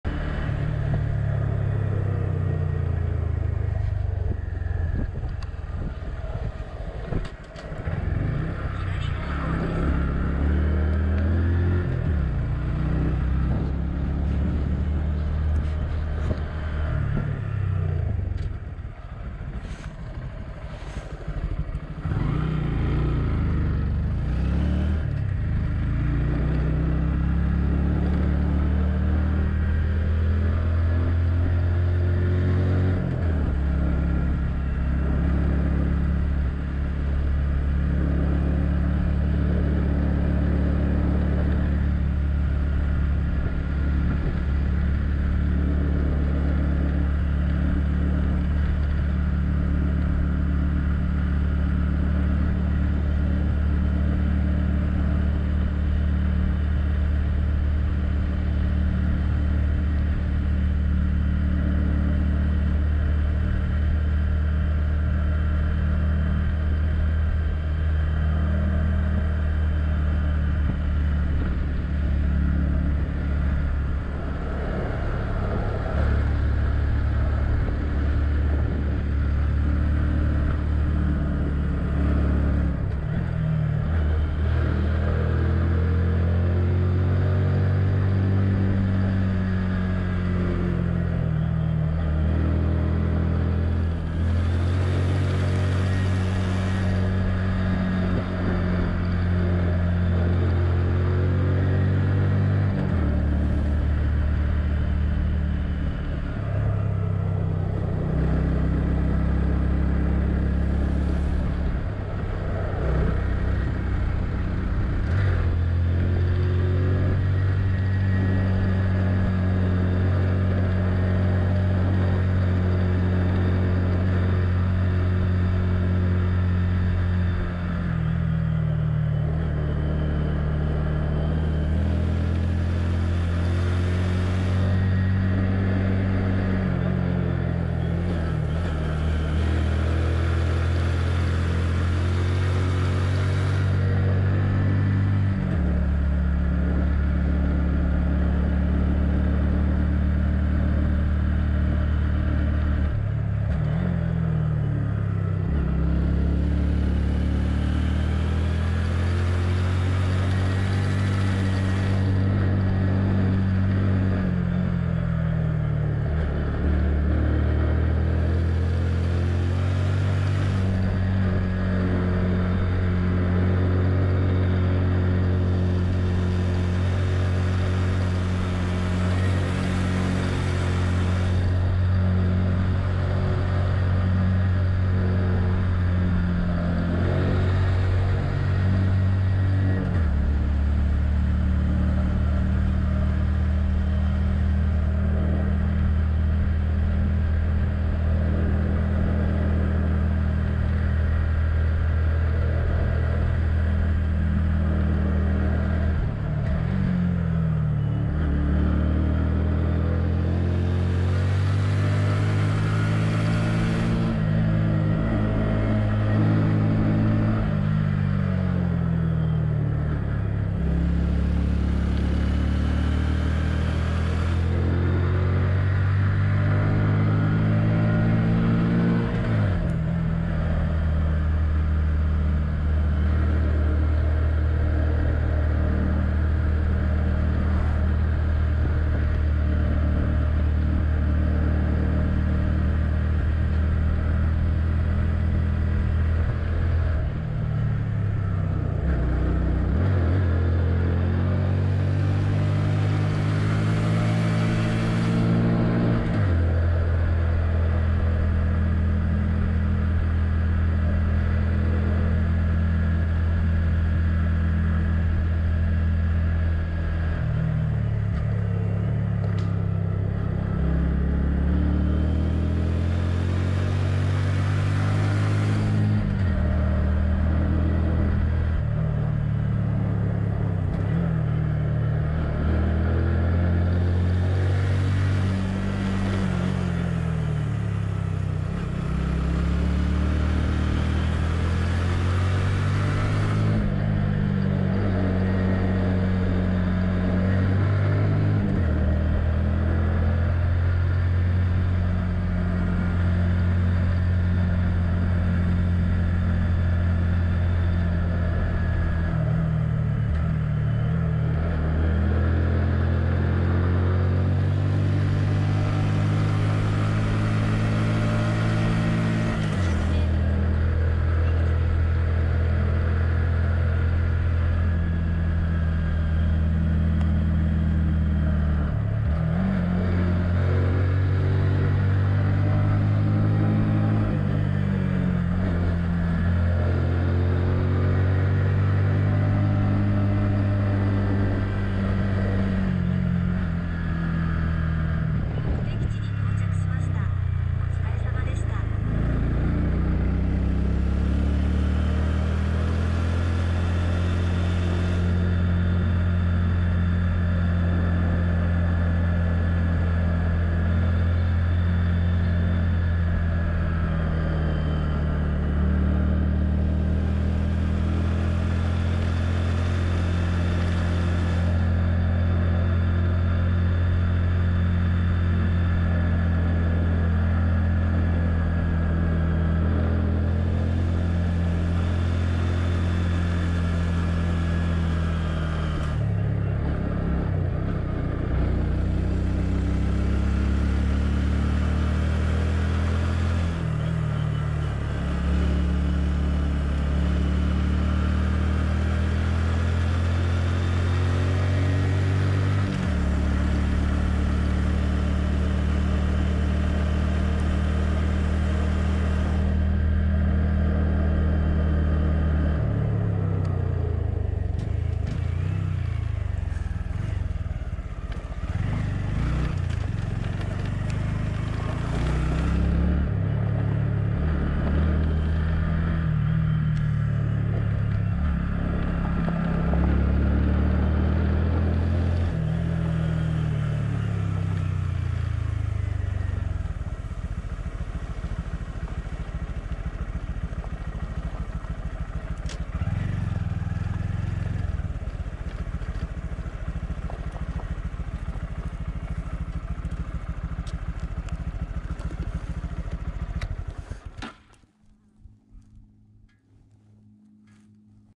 【GROM125】県道 険道448号～魚野テイクオフ場＠鹿児島 2020.4.14
距離★☆☆　路面荒れ★☆☆　難易度★☆☆　吉松中央公民館～魚野テイクオフ場
GoPro HERO7で撮影、県道448号川西菱刈線
総延長4キロ。